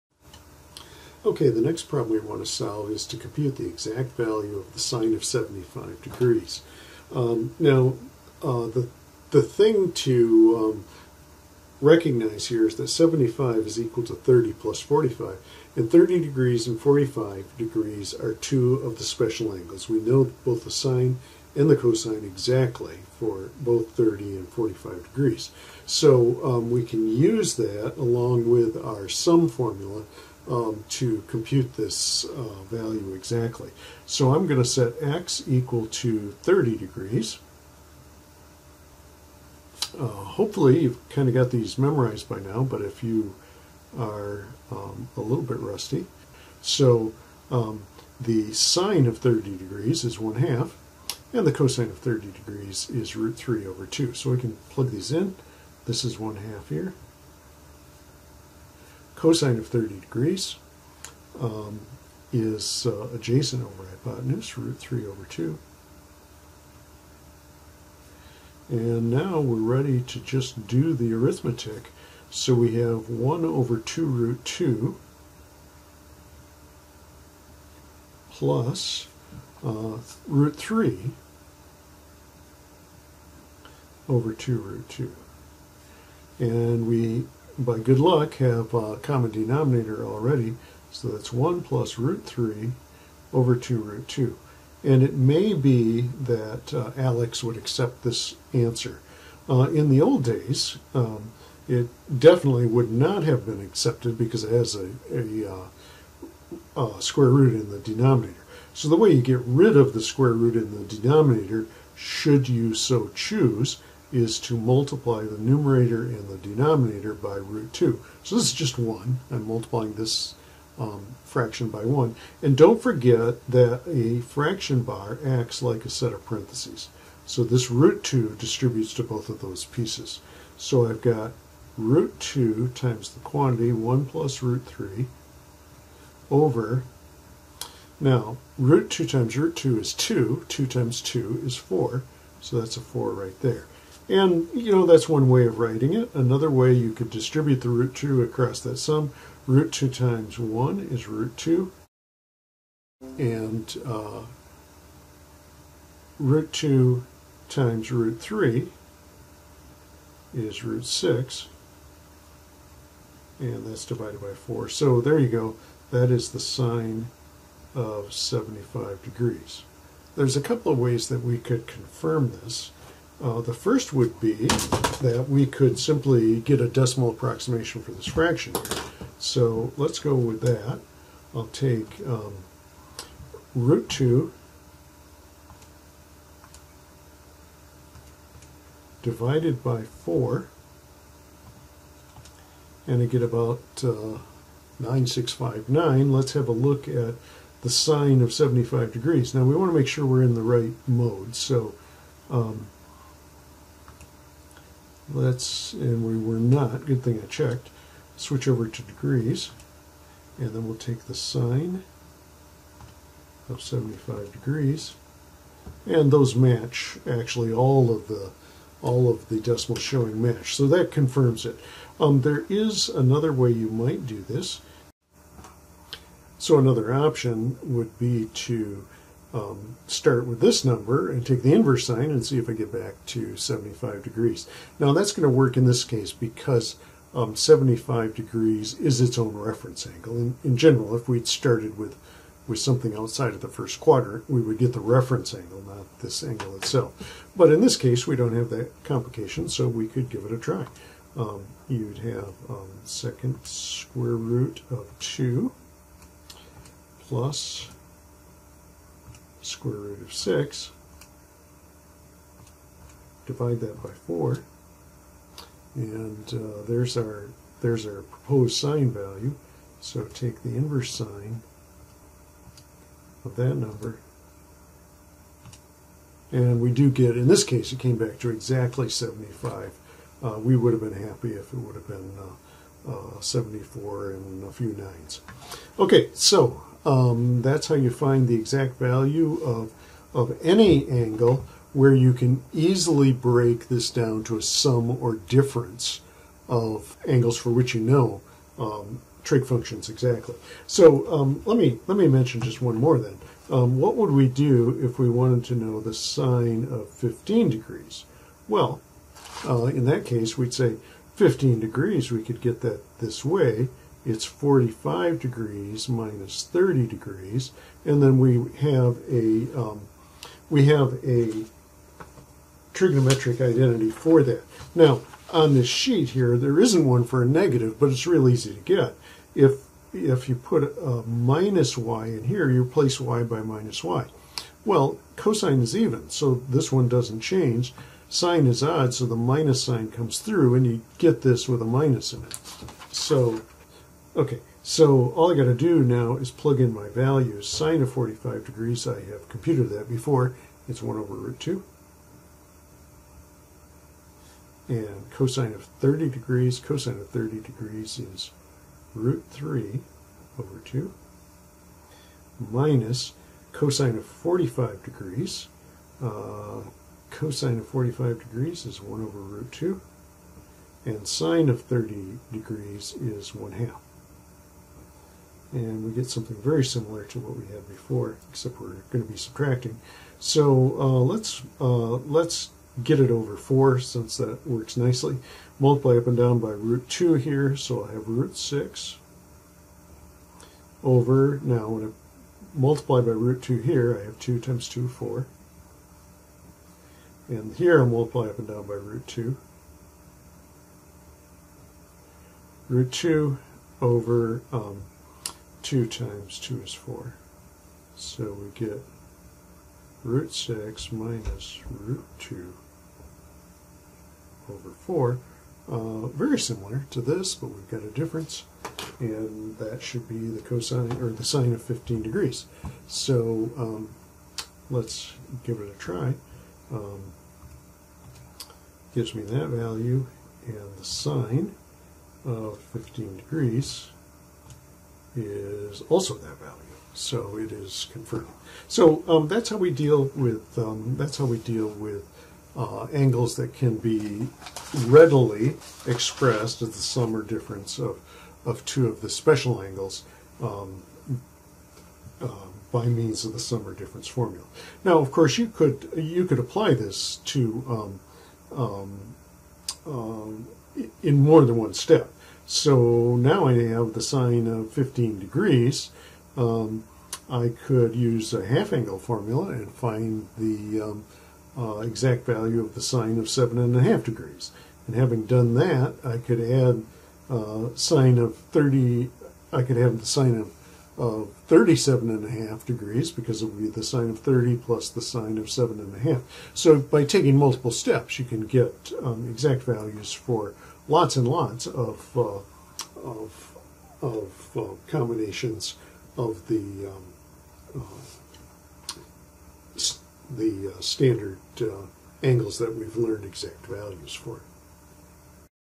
Exact value of sine 75 degrees
Computing the exact value of sin(75 degrees) and sin(15) degrees by using the addition and subtraction formulas for sine.